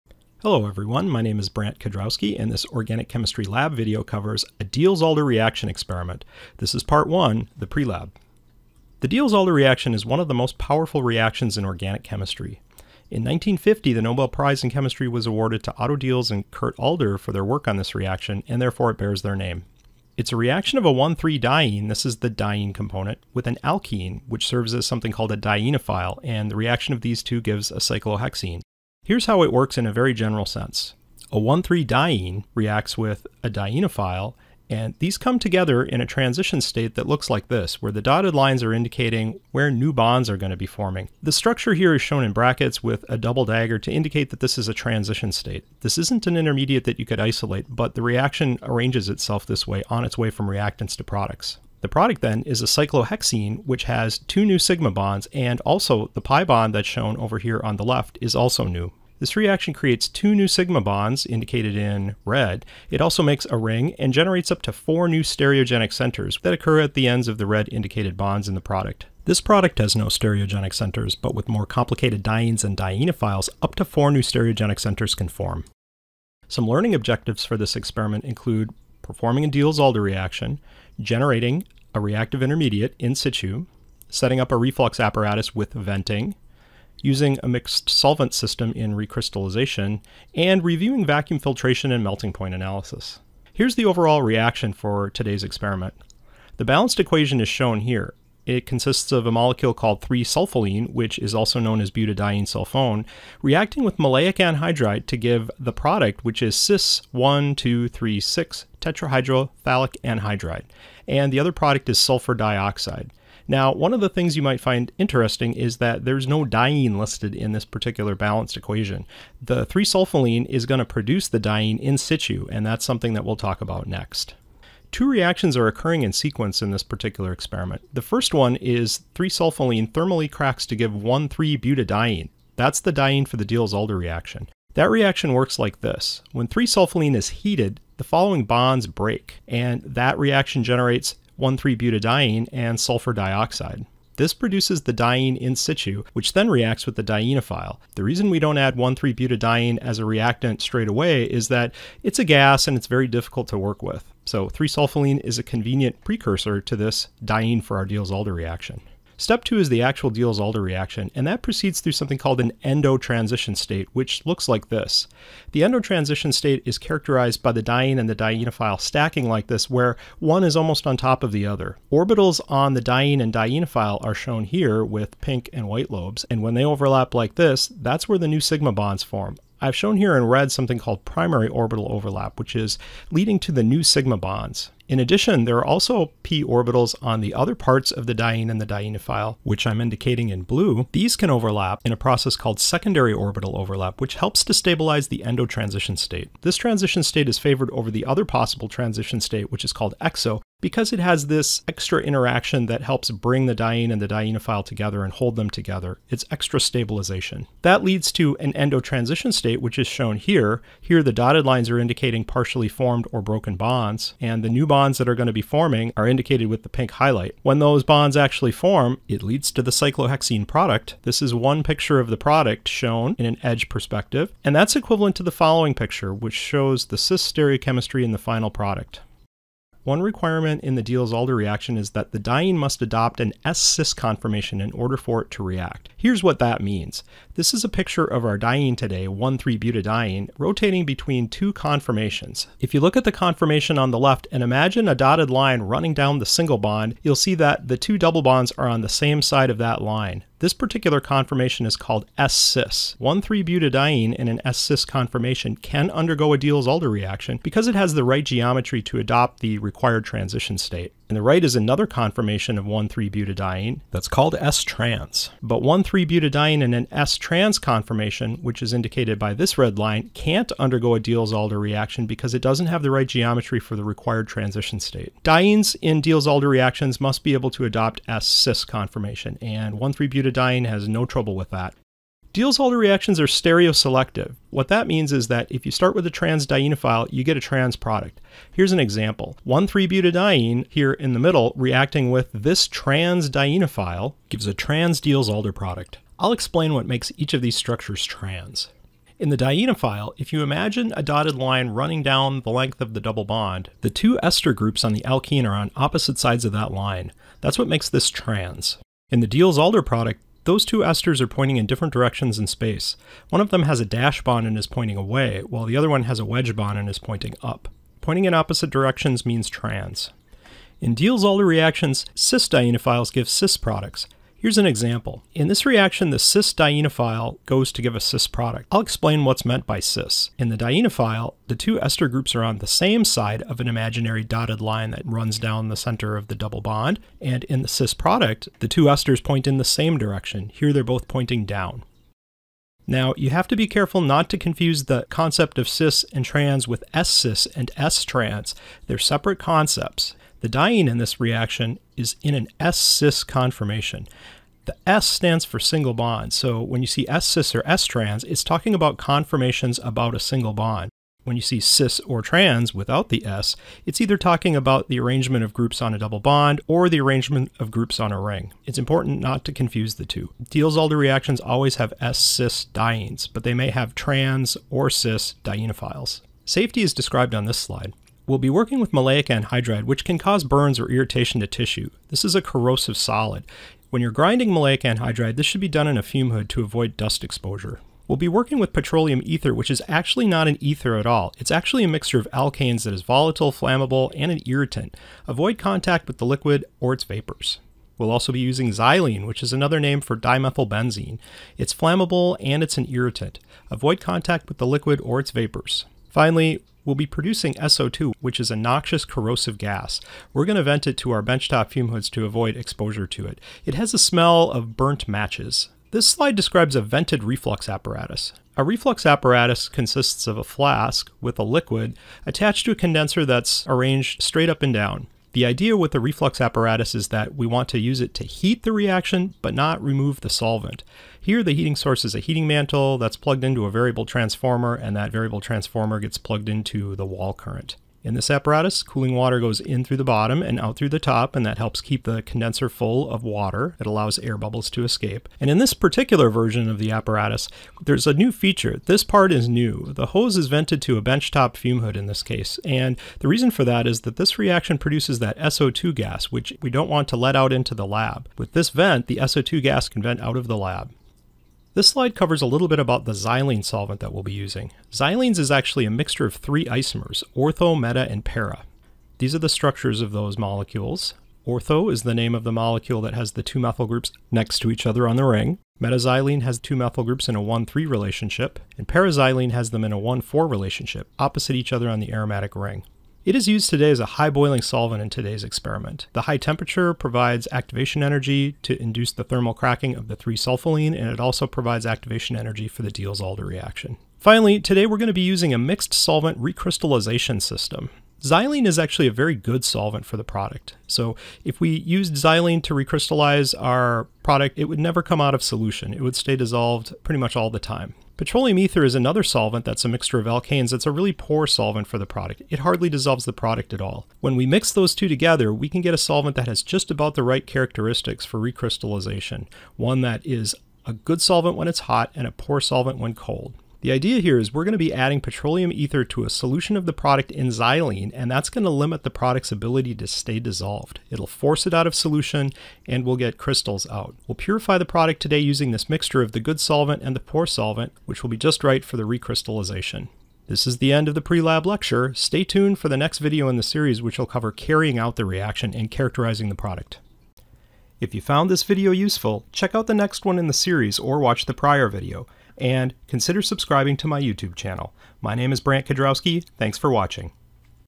Diels Alder Reaction Experiment Part 1, Prelab
This organic chemistry lab video describes a Diels-Alder reaction lab experiment.  The molecule 3-sulfolene (butadiene sulfone) cracks thermally to give 1,3-butadiene in-situ, which then reacts with the dienophile maleic anhydride.  The Diels-Alder product is cis-1,2,3,6-tertahydrophthalic anhydride.  The Diels-Alder reaction is discussed, including the requirement that the diene adopt an s-cis conformation.  The stereoselectivity of the reaction is also discussed along with the reaction's preference for an endo transition state.